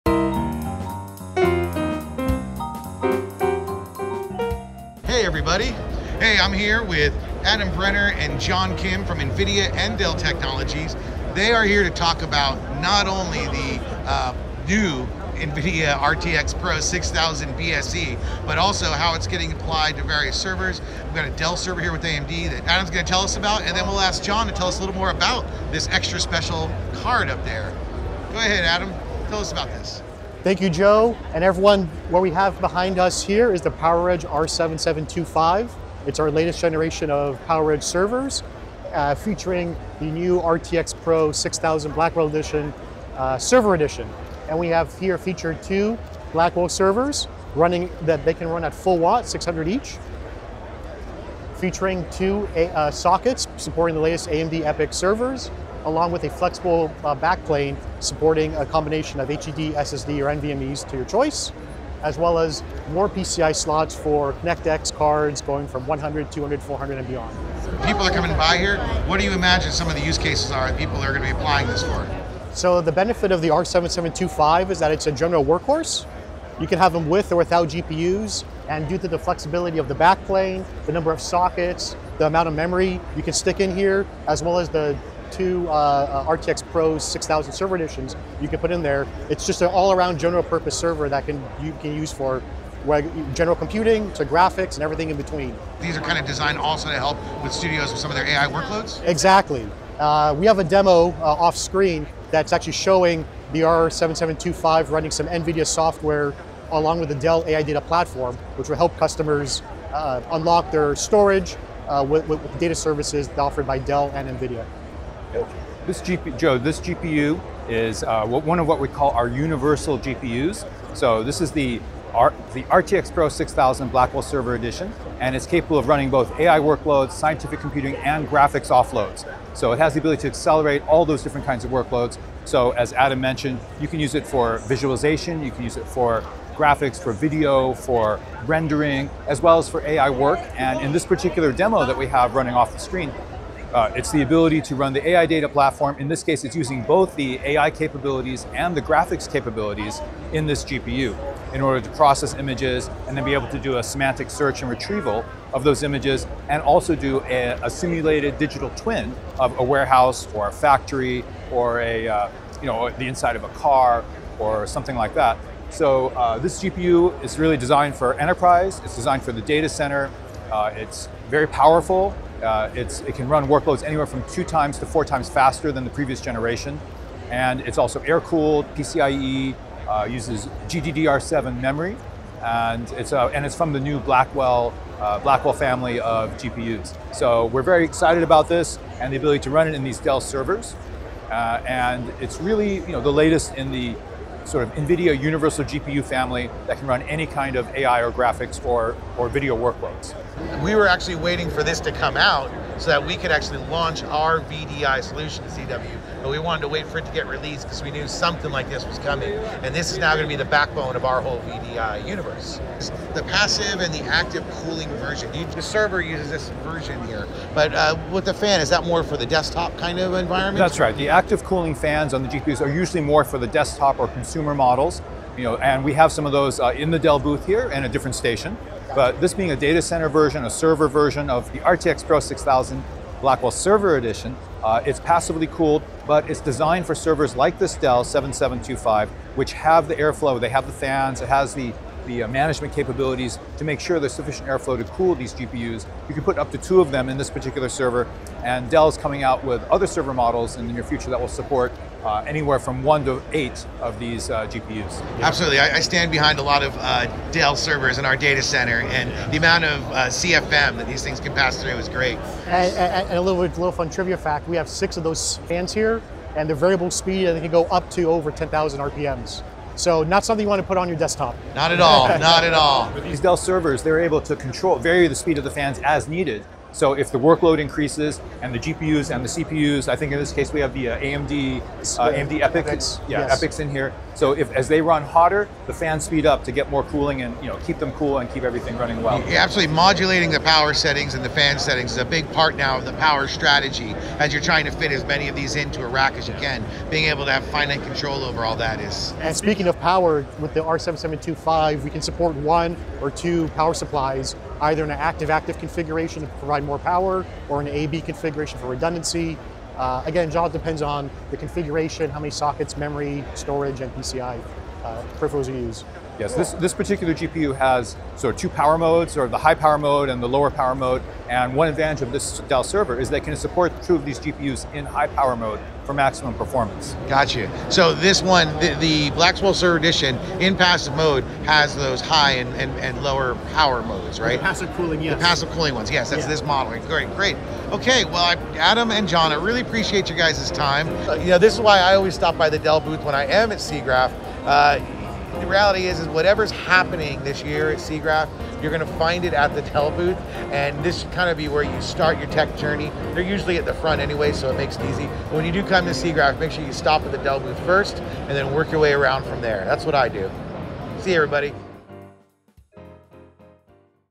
Newest NVIDIA RTX PRO 6000 BSE (2x GPUS = 192GB VRAM) Dell PowerEdge 7725, with 2x AMD CPU 384 Cores
At SIGGRAPH2025, we caught up with Adam Brenner from @DellTechnologies and John Kim from @NVIDIA take a deep dive into the new Nvidia RTX Pro 6000 Blackwell Server Edition GPU. Hosted inside a Dell PowerEdge R7725 server, this powerhouse setup combines the flexibility of Dell’s latest enterprise hardware with the strength of @AMD EPYC processors. Together, they unlock the ability to run AI workloads, high-end graphics, scientific computing, and enterprise data platforms at remarkable speeds.

I was particularly interested in seeing this server since it is targeted as the platform for our new CDW StudioCloud VDI offering while also a workhorse for AI, GPU or real-time workloads in VFX & Animation

In this walkthrough, you’ll see how Dell, Nvidia, and AMD are working together to deliver scalable solutions for studios, enterprises, and data centers. From flexible storage backplanes to universal GPUs capable of powering visualization, rendering, AI training, and even digital twin simulations, this tech demo highlights why the Dell booth at SIGGRAPH is always a must-visit. Whether you’re building out AI infrastructure, optimizing VDI solutions, or pushing the limits of performance, this Dell + Nvidia + AMD collaboration shows the future of enterprise computing.